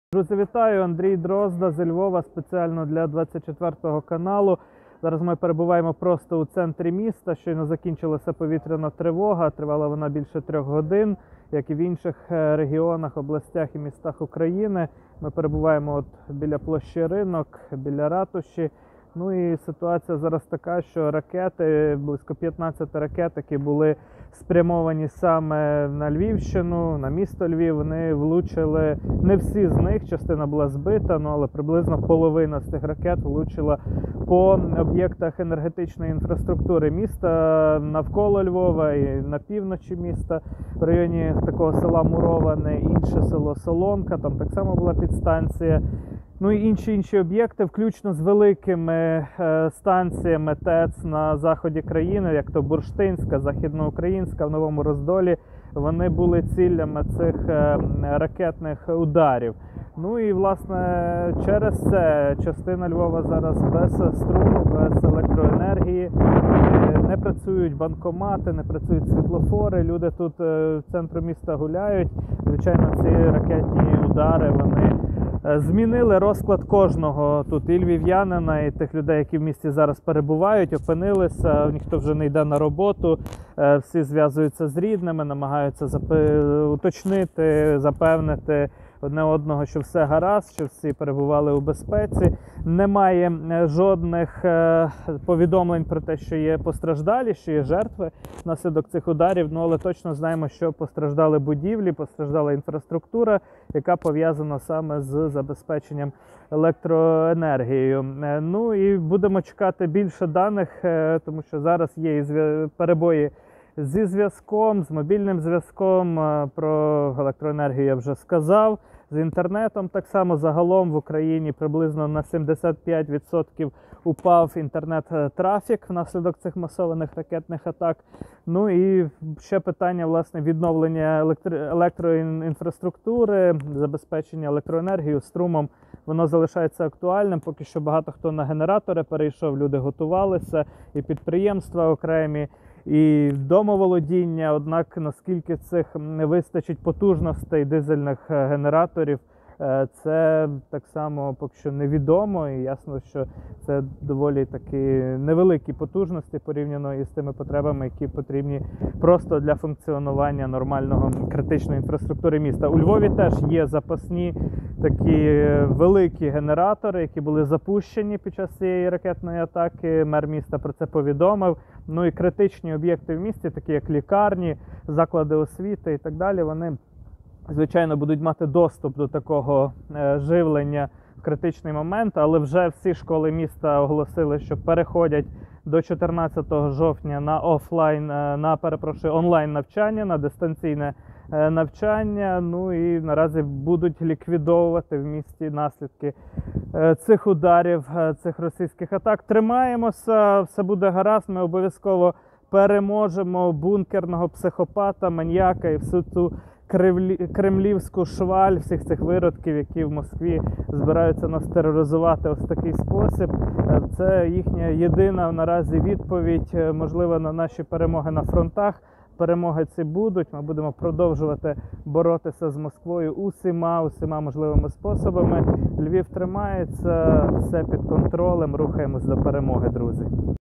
Львів після ранкових прильотів || Ситуація у місті
Про ситуацію у Львові після ранкових масованих ударів розповів ведучий Андрій Дрозда.